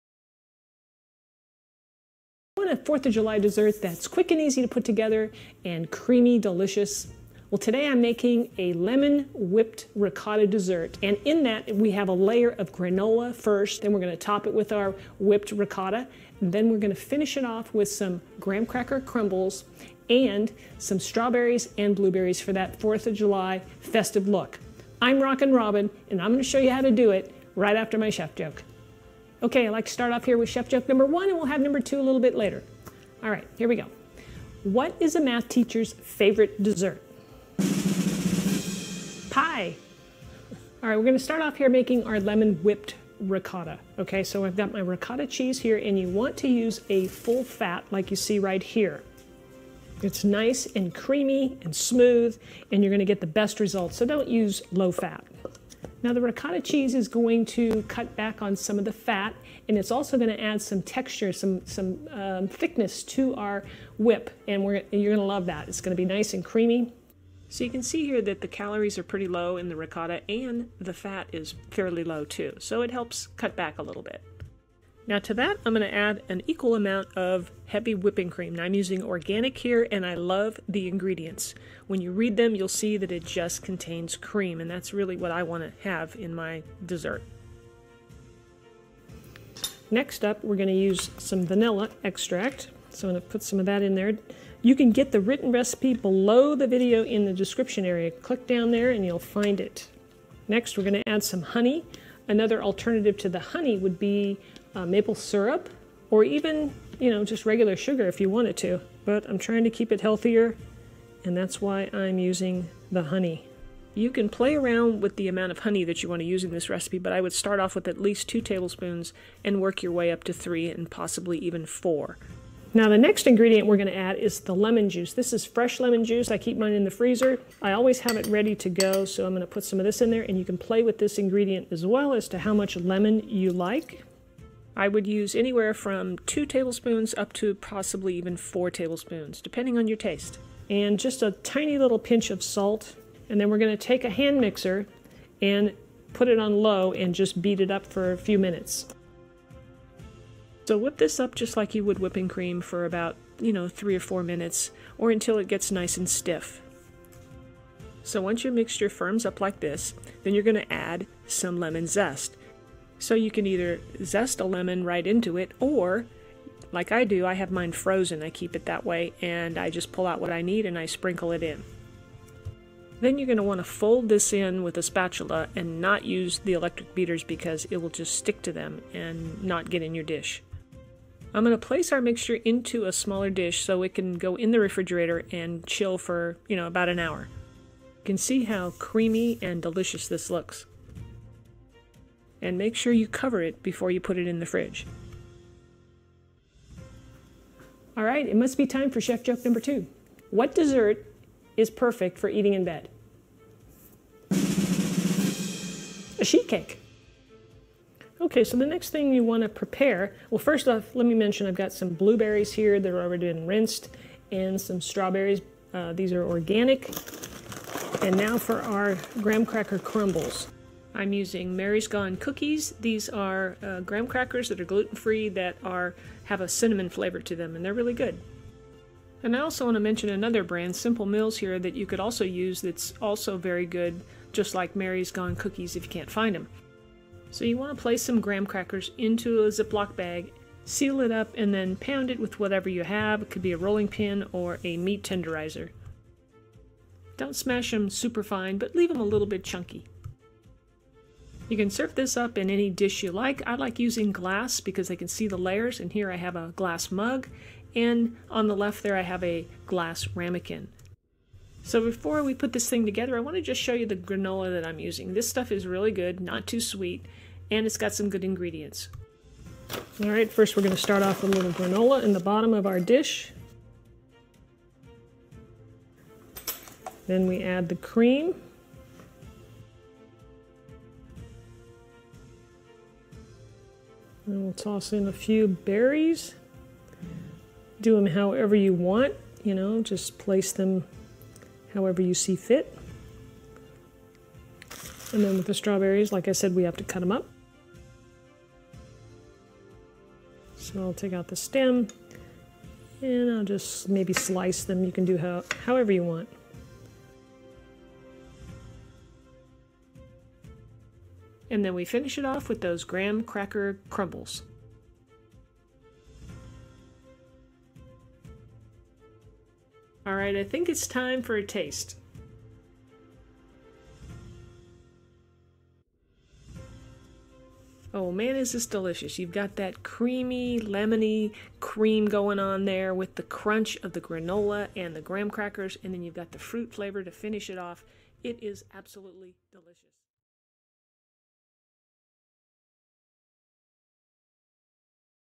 Red White and Blue Lemon Whip Ricotta Parfait | 4th of July DIY Snacks!
Check out this easy to make and easy to follow recipe from Rockin Robin Cooks!  This is a tasty and refreshing dish that can be served at your next Fourth of July Themed Party!